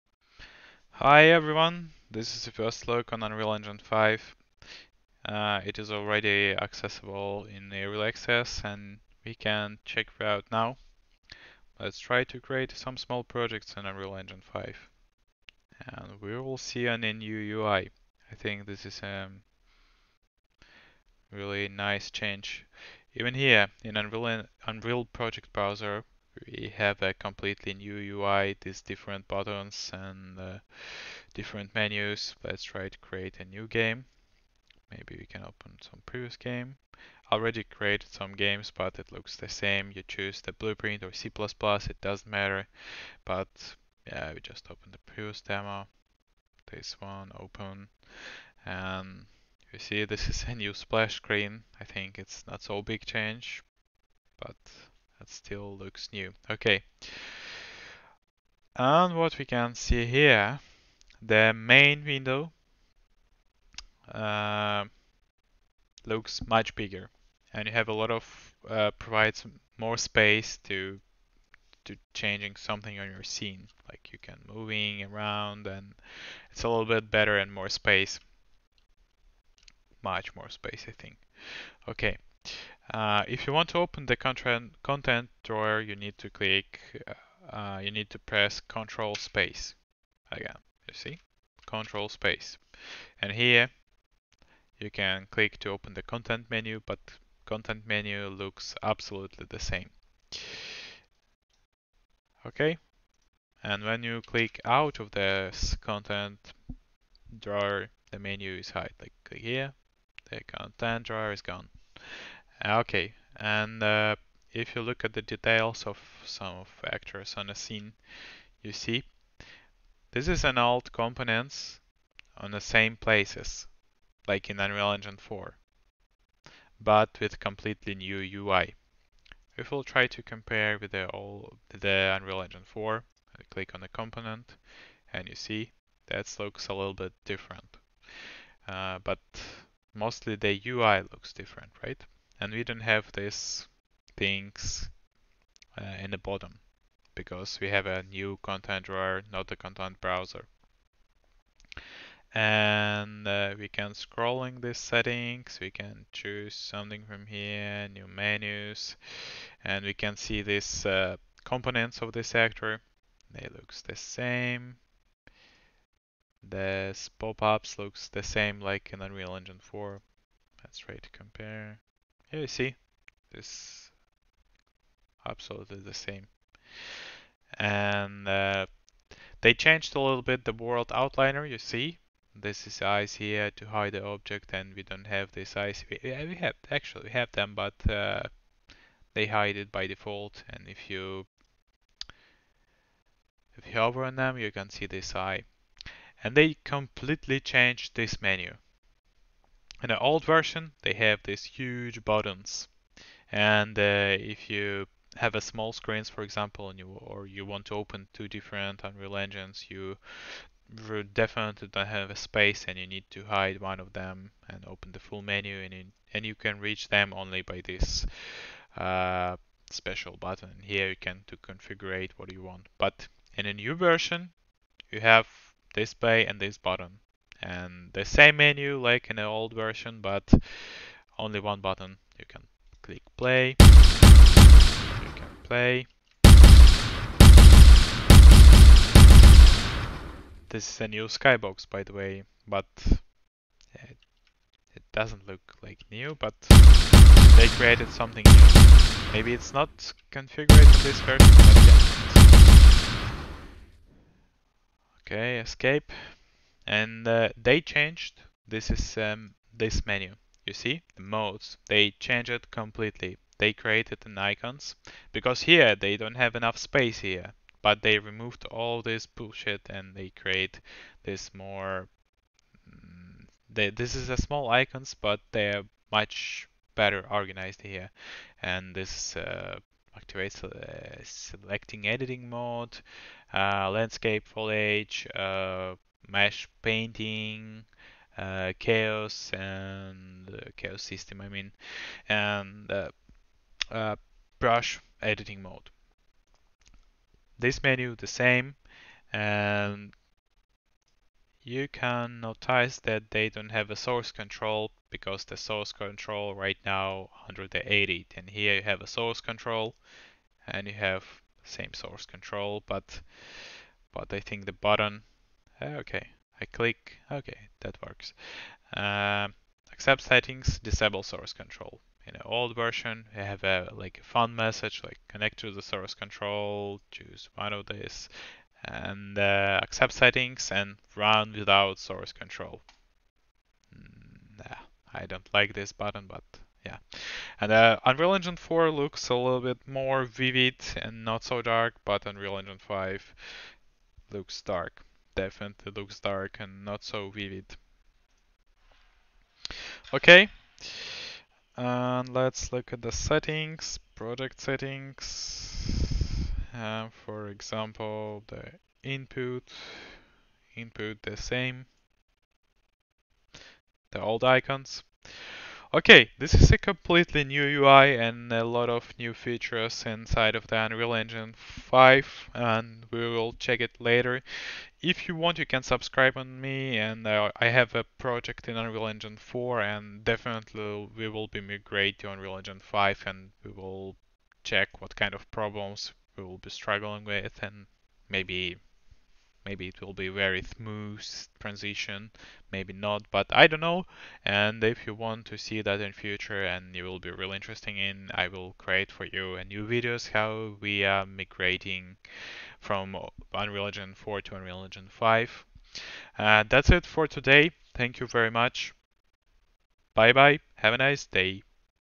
UE5 vs UE4 - UI comparison
This is the first look at UE5. I'm comparing the new UI with the old UI in UE4 and provides my thought about it.